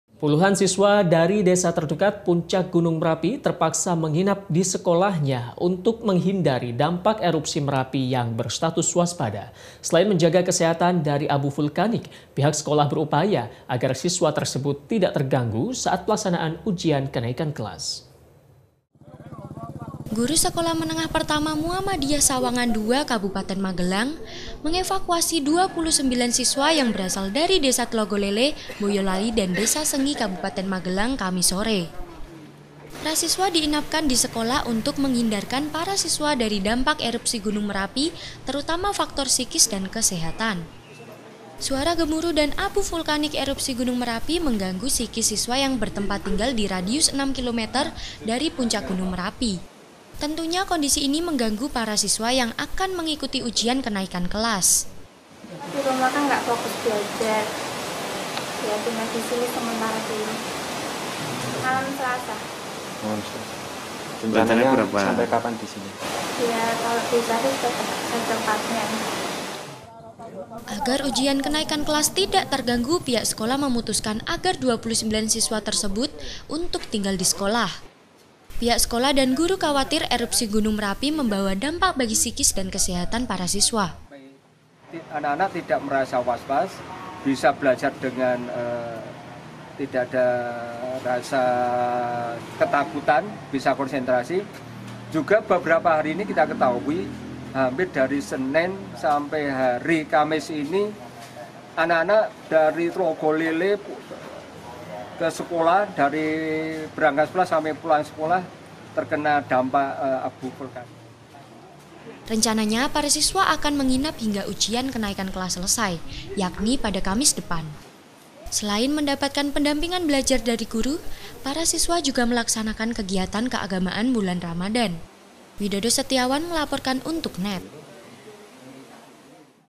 Hindari Bencana Erupsi Merapi, 29 Siswa Menginap di Sekolah - NET JATENG
Guru Sekolah Menengah Pertama Muhammadiyah Sawangan Dua, Kabupaten Magelang, mengevakuasi dua puluh sembilan siswa yang berasal dari Desa Tlogolele, Boyolali, dan Desa Sengi, Kabupaten Magelang , kamis sore. Para siswa diinapkan di sekolah, untuk menghindarkan para siswa dari dampak erupsi Gunung Merapi, terutama faktor psikis dan kesehatan.

Suara gemuruh dan abu vulkanik erupsi Gunung Merapi, mengganggu psikis siswa yang bertempat tinggal di radius enam kilometer dari puncak Gunung Merapi. Tentunya, kondisi ini mengganggu para siswa, yang akan mengikuti ujian kenaikan kelas.

Agar ujian kenaikan kelas tidak terganggu, pihak sekolah memutuskan agar 29 siswa tersebut untuk tinggal disekolah. Pihak sekolah dan guru khawatir erupsi Gunung Merapi membawa dampak bagi psikis dan kesehatan para siswa.

“Tidak merasa was-was, bisa belajar dengan tidak ada rasa ketakutan, bisa konsentrasi, juga beberapa hari ini kita ketahui dari hari senin sampai hari kamis ini anak-anak dari tlogolele ke sekolah, dari berangkat sekolah, terkena dampak abu vulkanik.” Ujar AHMAD TAUFIK – Guru.

Rencananya, para siswa akan menginap hingga ujian kenaikan kelas selesai, yakni pada kamis depan. Selain mendapatkan pendampingan belajar dari guru, para siswa juga melaksanakan kegiatan keagamaan Bulan Ramadhan.